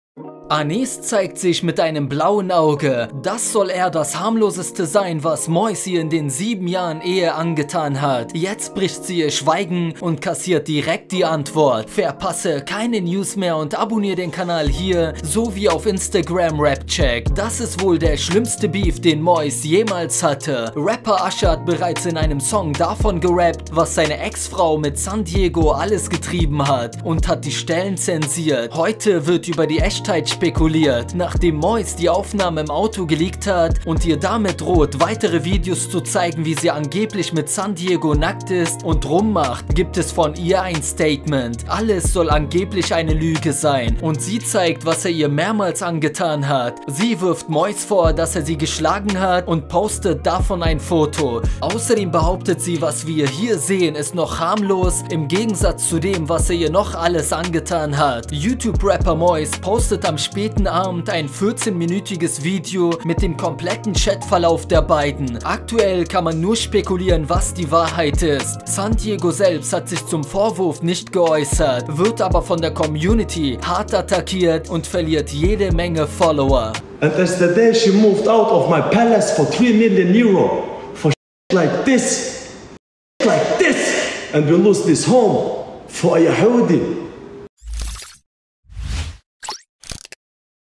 ANYS will MOIS zerstören & zeigt Fotos wie er sie geschlagen hat - VORWURF!
Quelle: Mois & Sun Diego